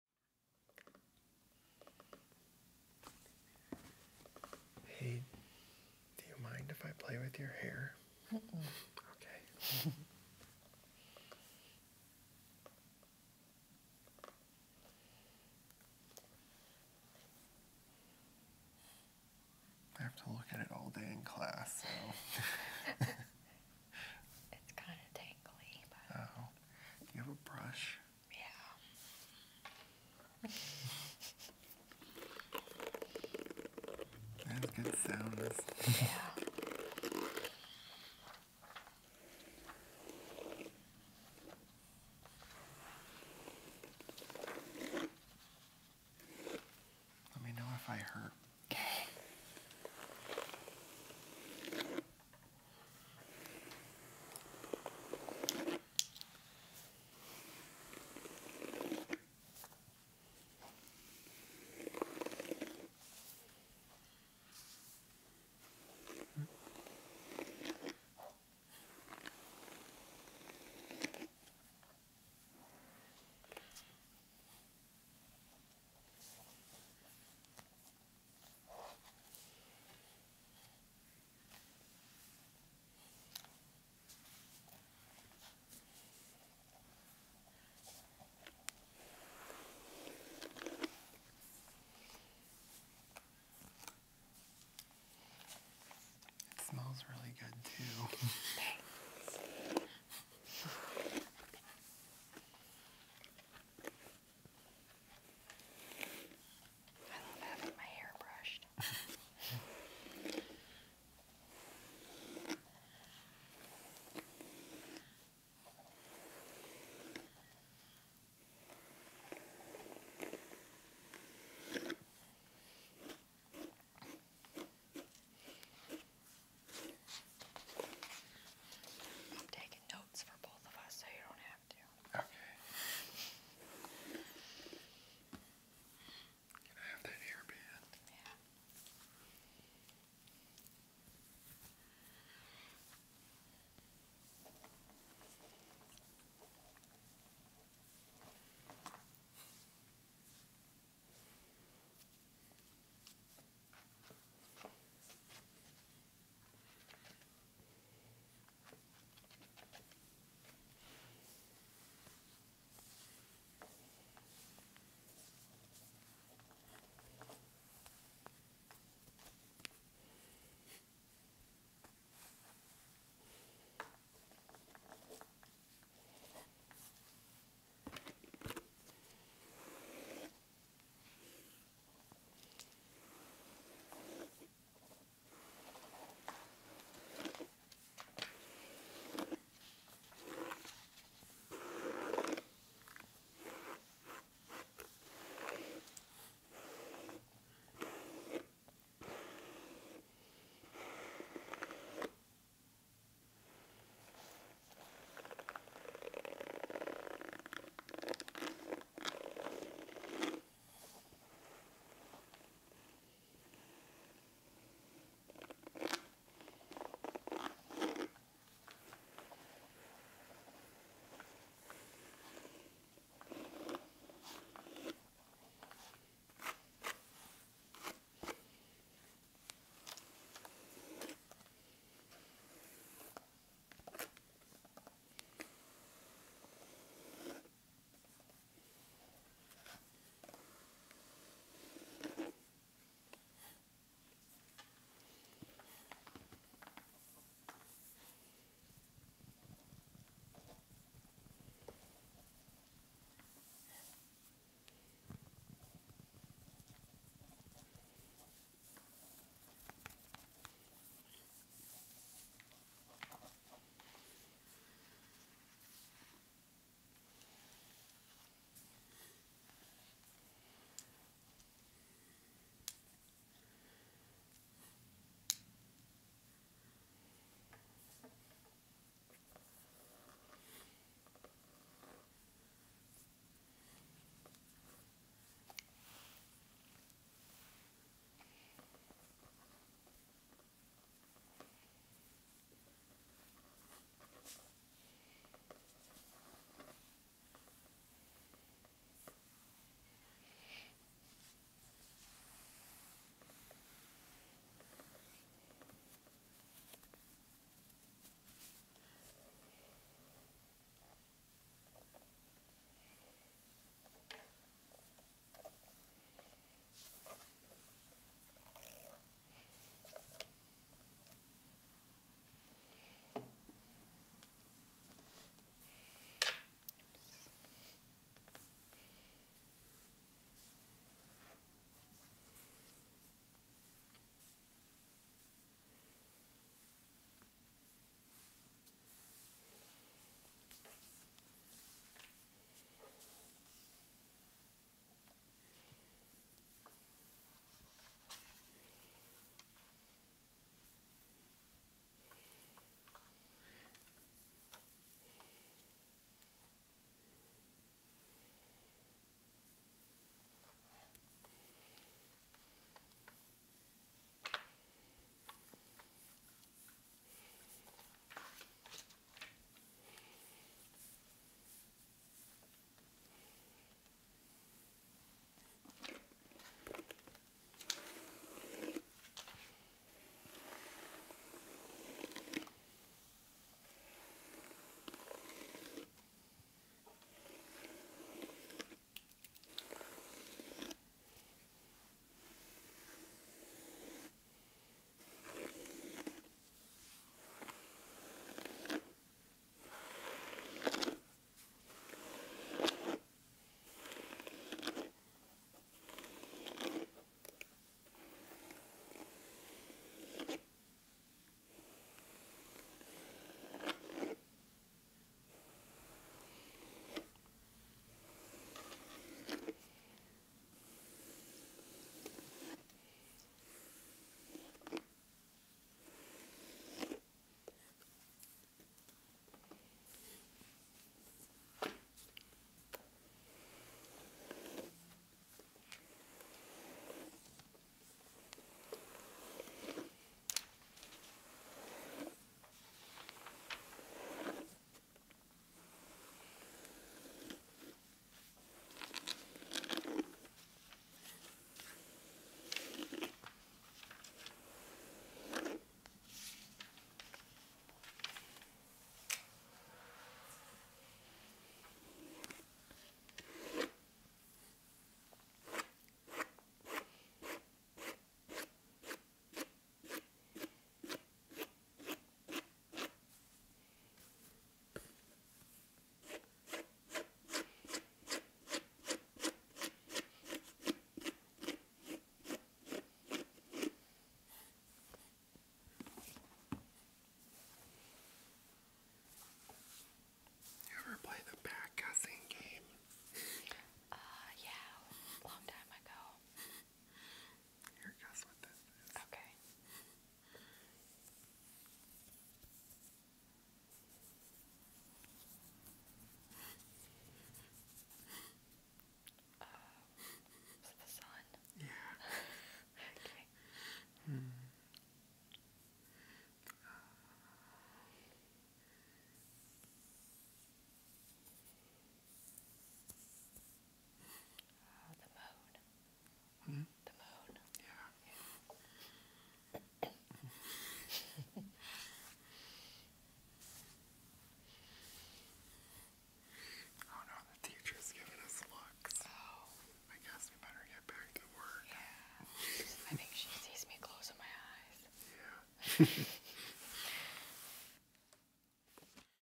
ASMR | Guy in study hall plays with your hair 😴 | Hair brushing, back tickles, soft spoken 🩵
✨Study hall role play✨

Relax with this tingly hair brushing ASMR session.

Hair play, hair brushing, brushing the back, ponytail brushing, and some back tracing/guessing game.

Soft spoken, notebook and paper sounds, writing/taking notes.

Hope you enjoy and this helps you relax and fall asleep fast 😴